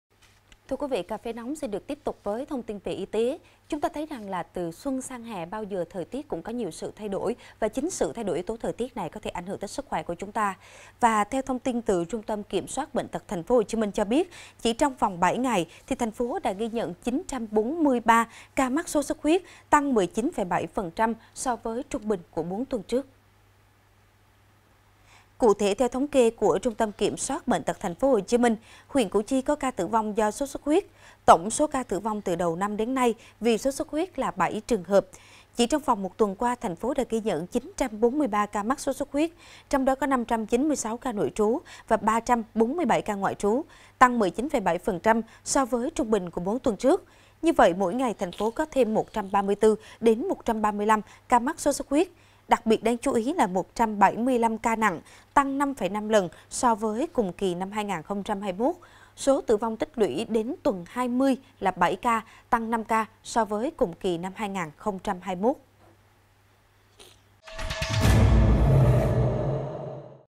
TP HCM: XUẤT HIỆN THÊM 79 Ổ DỊCH SỐT XUẤT HUYẾT MỚI | VTC9
VTC9 | TP HCM: XUẤT HIỆN THÊM 79 Ổ DỊCH SỐT XUẤT HUYẾT MỚI